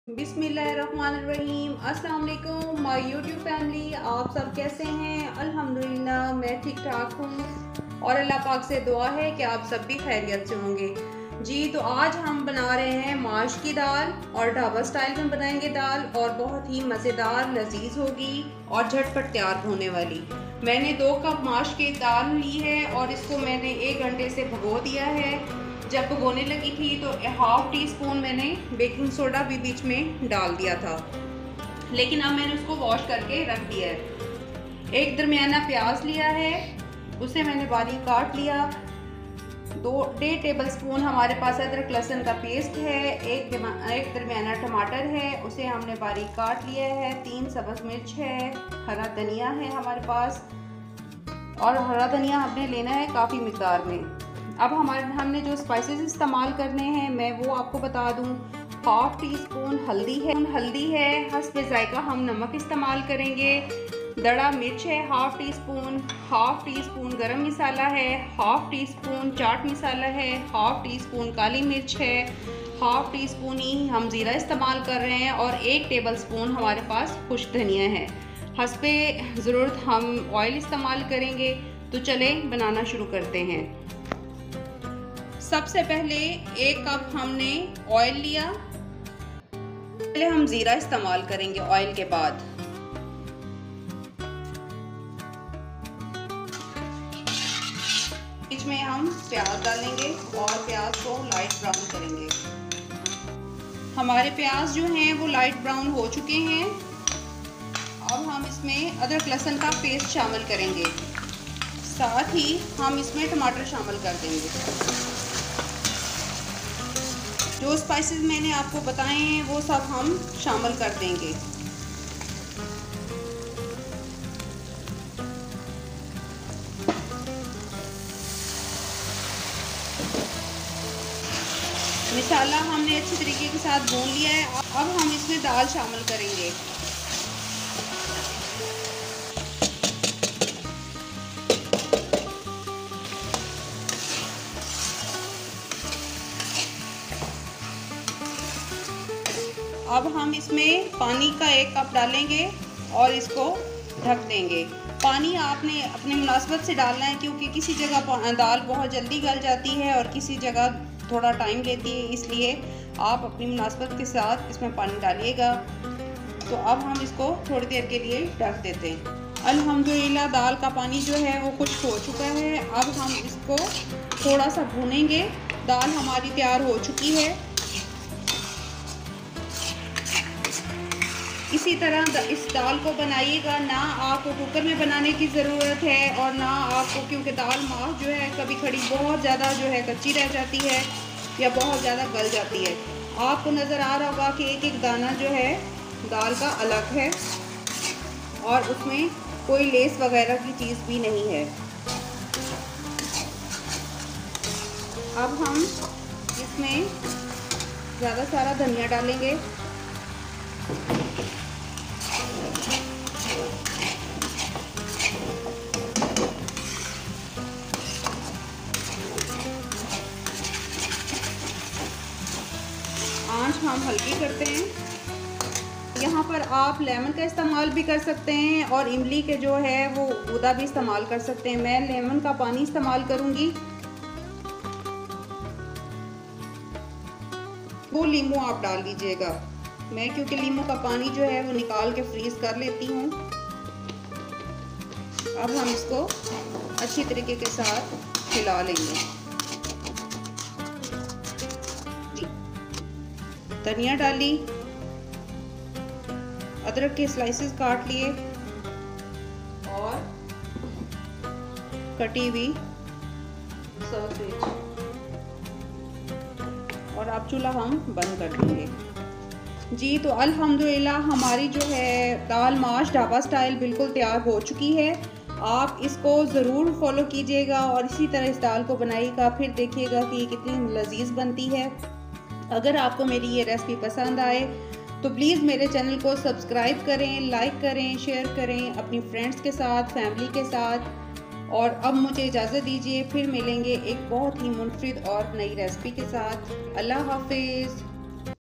Kaka khail daal Mash / Dhaba style#mindblowing#deliciousrecipe#easy#quick
Hope u all like my videooo. ❤
I will tell u how u will make quick recipies .
today i make kaka khail daal mash ...
dhaba style .u must try this recipe ...
i will give u more interesting videos related to food .